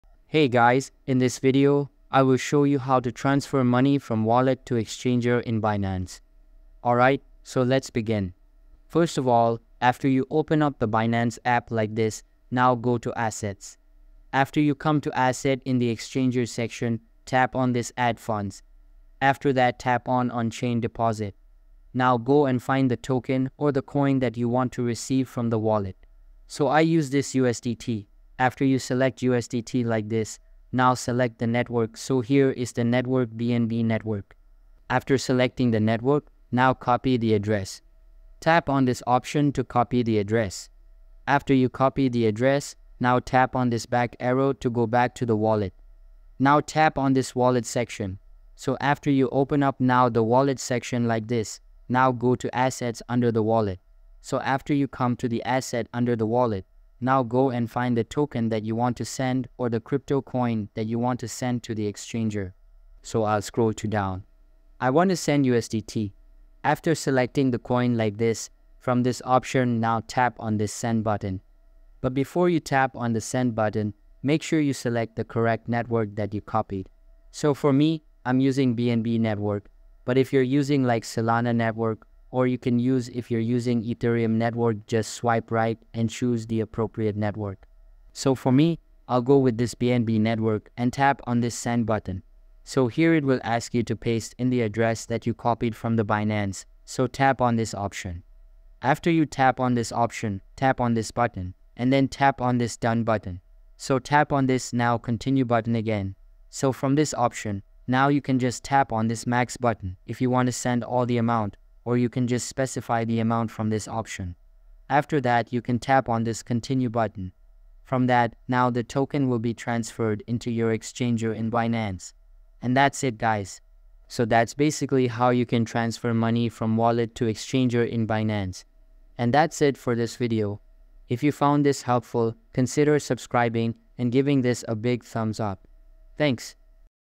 How to transfer money from wallet to exchange binance
In this video I'll show you How to transfer money from wallet to exchange binance. The method is very simple and clearly described in the video. Follow all of the steps & How to transfer money from wallet to exchange binance

إذا كان هذا الفيديو قد ساعدك، يرجى الإعجاب بالفيديو وترك تعليق في الأسفل. شكرًا!